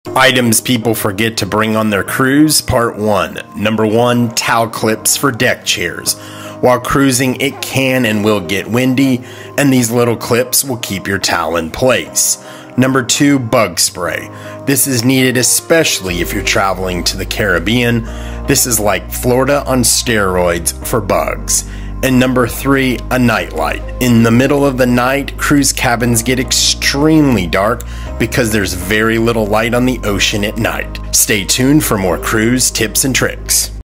Things people FORGET 🤷‍♂️ to bring on their cruise 🚢 part 1
Items people forget to bring on their cruise part one

This will be a series of small things that people forget to bring on their cruise and can be packed very easily, and they can make your cruise go much smoother.

These are tips and tricks that we’ve picked up in our 30+ cruises that we wish we would’ve known sooner so learn from our mistakes.

Currently, we are at 17,000 subscribers and growing , I appreciate everyone support.